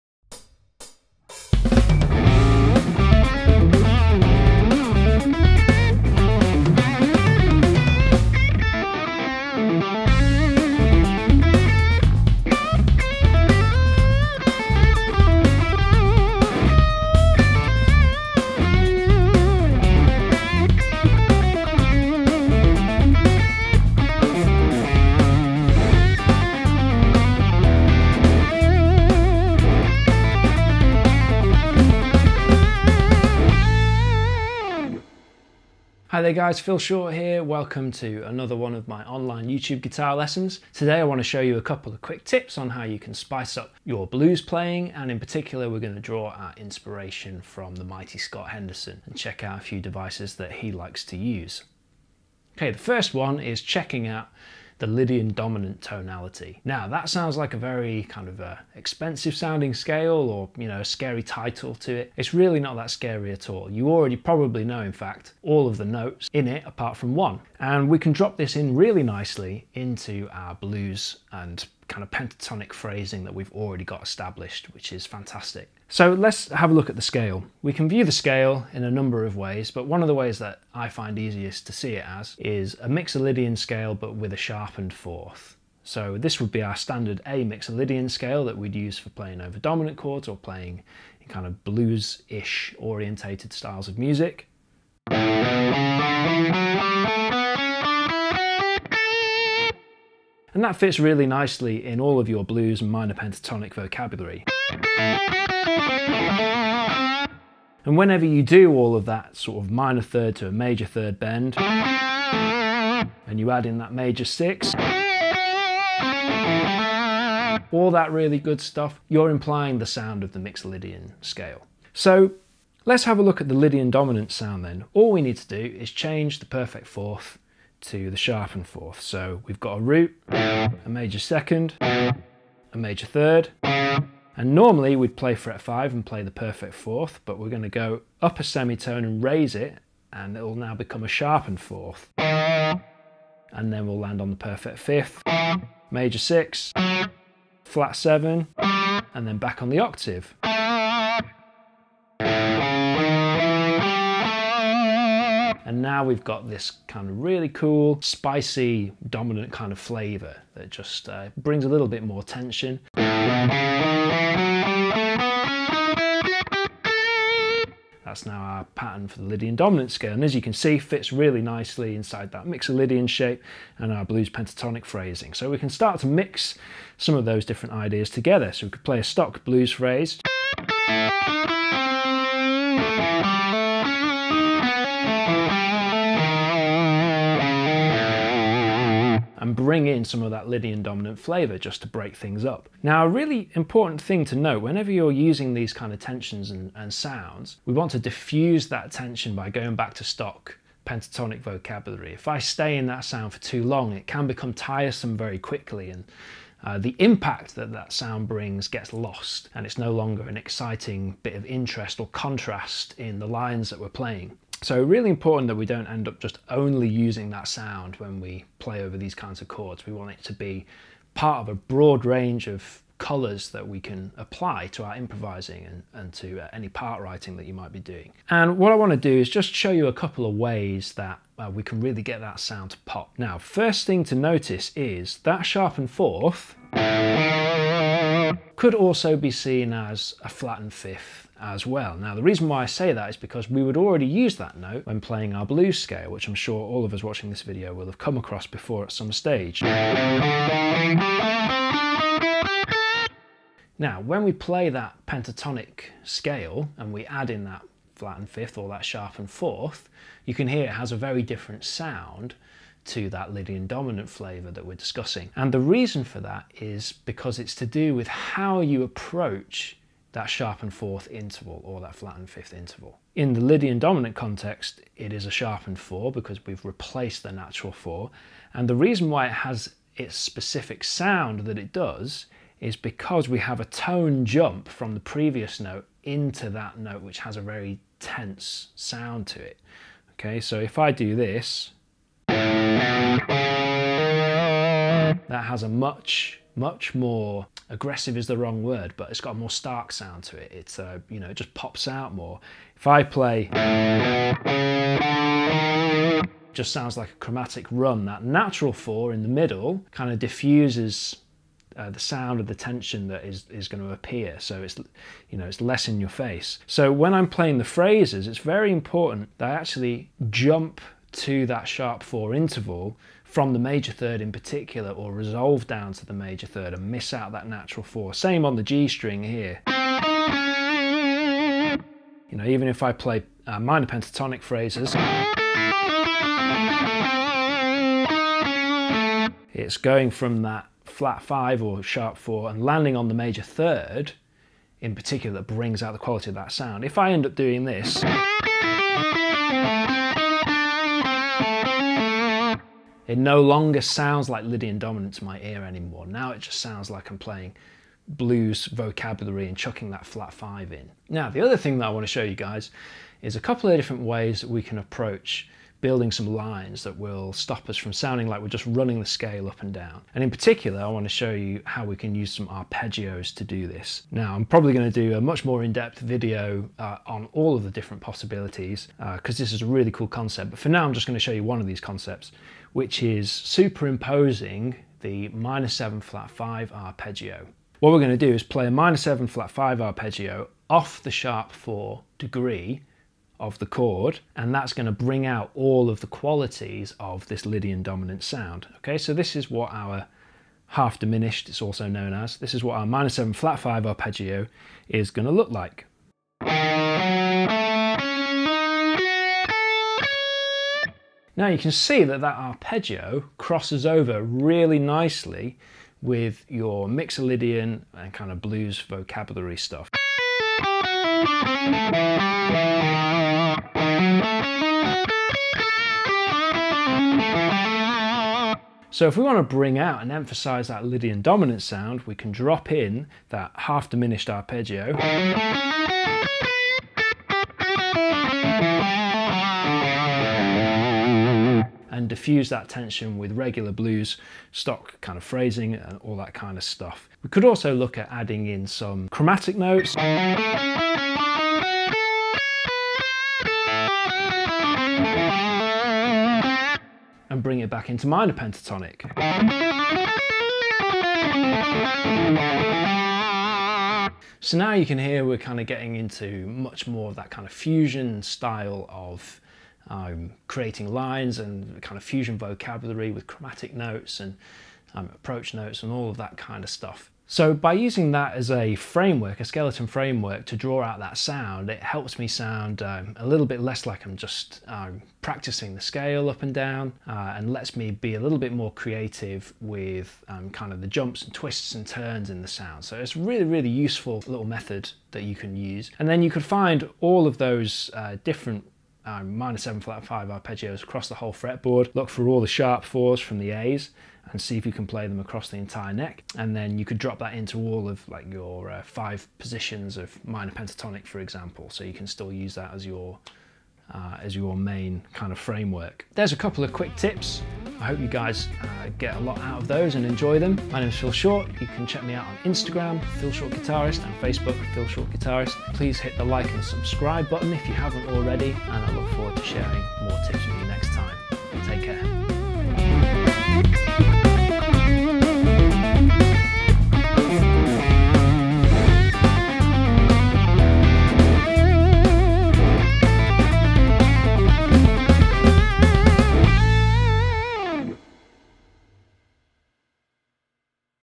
How to sound like: Scott Henderson
This week we take a look at a few quick tips that will help you get into the sound of one of the most musical blues/rock/fusion giants of guitar; the mighty Scott Henderson!

Check me out on Instagram and facebook @philshortguitarist